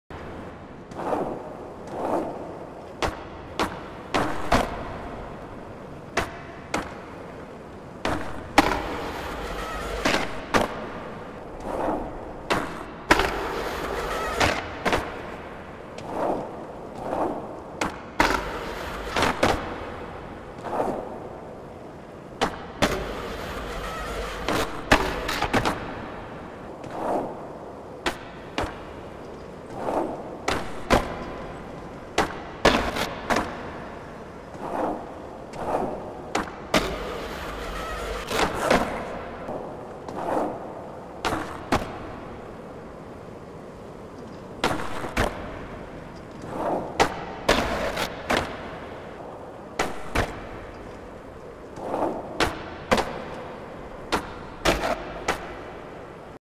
Session Indoor Park 2.0 Tape #4 (Them Bleachers)
Sessioning the bleachers is fun! look at the wobble on the trucks haha.